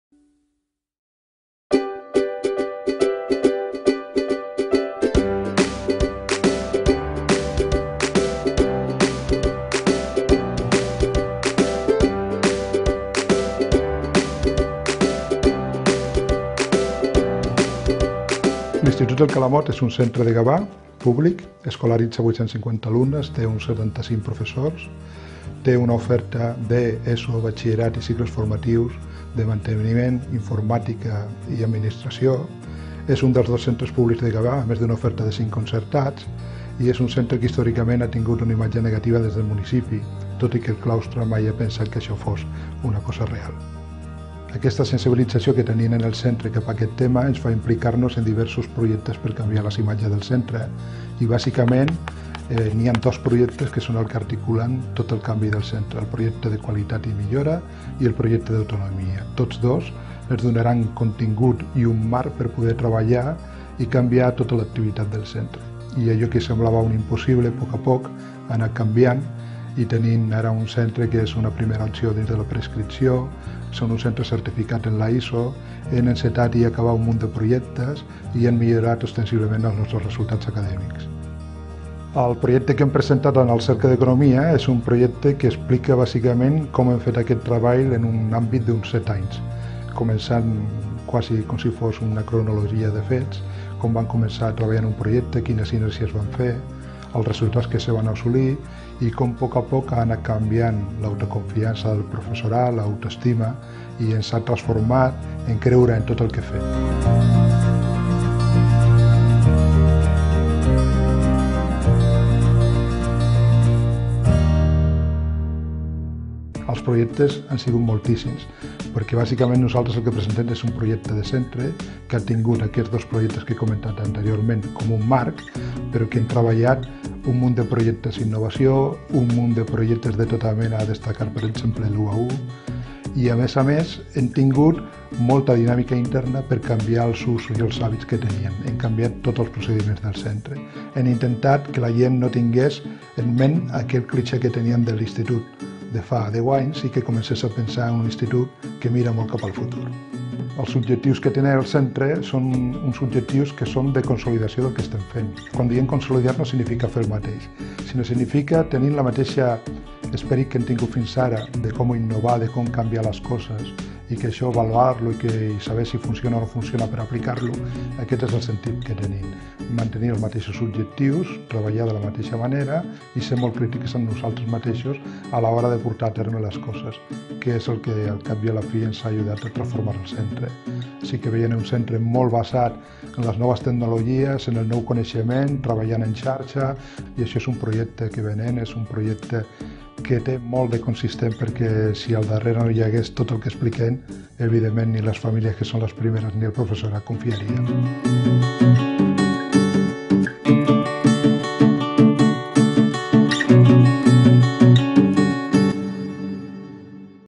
INS El Calamot: Primer Premi Ensenyament Fundació Cercle d'Economia
Vídeo de Presentació de l'INS El Calamot als Premis Ensenyament del Cercle d'Economia,  en el que ha resultat guanyador. dimarts 8 de maig de 2012